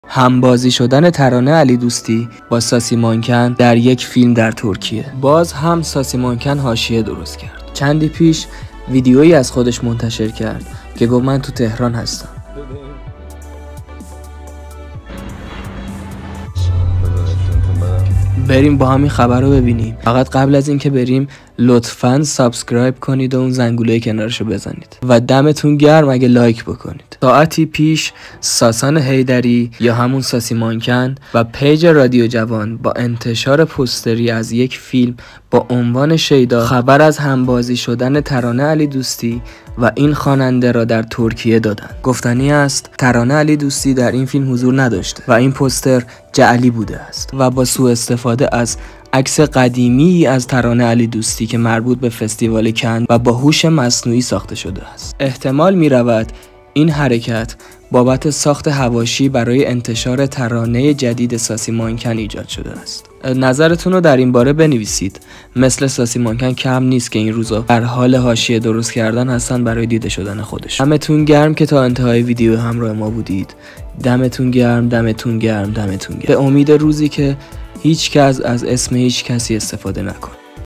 همبازی شدن ساسی مانکن با ترانه علیدوستی در فیلم شیدا! حاشیه
همبازی شدن ساسی مانکن با ترانه علیدوستی در فیلم شیدا
خبر جعلی همبازی شدن ترانه علیدوستی با ساسی مانکن
عکس ترانه علیدوستی قدیمی است
بچند ساعتی است که ساسان حیدری یافته (ساسی مانکن) و پیج رادیو جوان با انتشار پوستری از یک فیلم با عنوان «شیدا» خبر از همبازی شدن ترانه علیدوستی و این خواننده در ترکیه داده‌اند.
گفتنی است ترانه علیدوستی در این فیلم حضور نداشته و این پوستر جعلی بوده و با سواستفاده از عکسی قدیمی از ترانه علیدوستی مربوط به فستیوال کن و با هوش مصنوعی ساخته شده است.
احتمال می‌رود این حرکت بابت ساخت حواشی برای انتشار ترانه جدید ساسی مانکن ایجاد شده باشد.